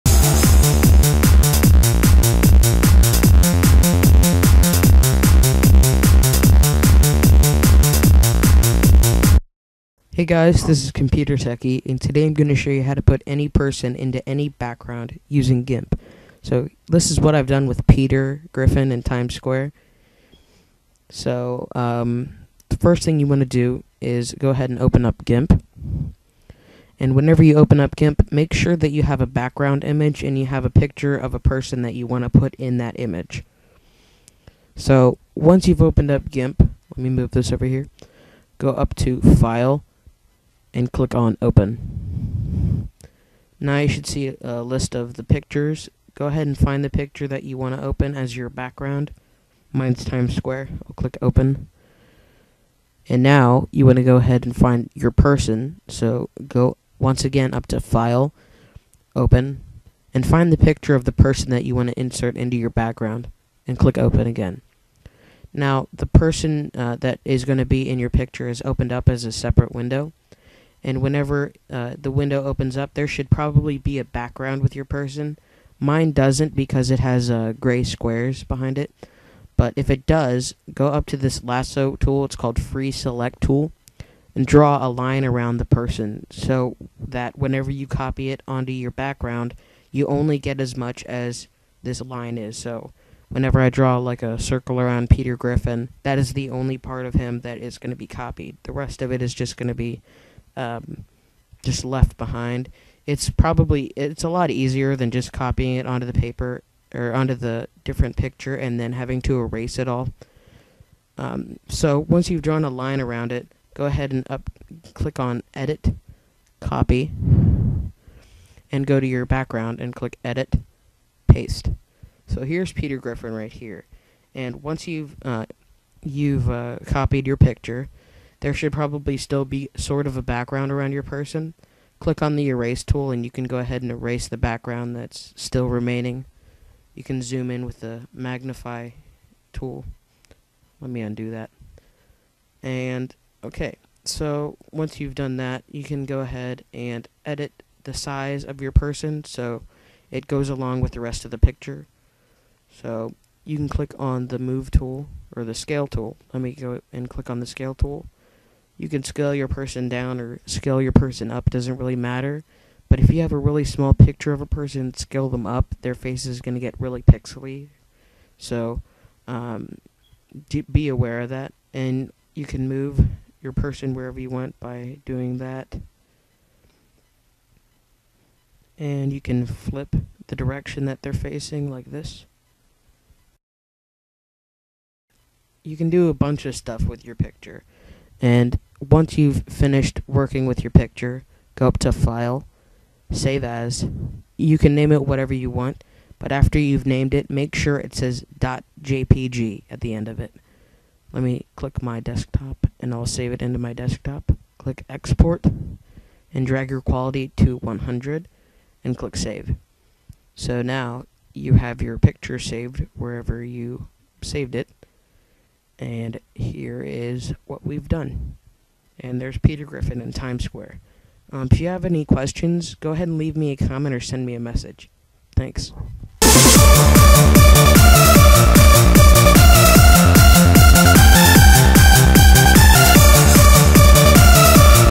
How to put a Person into a customizable background using GIMP
This video explains how to take a picture of any person, and put it into any customizable background.  In this tutorial, I put Peter Griffin in Times Square.  Hope you enjoy!

Intro/Outro Music by NAL1200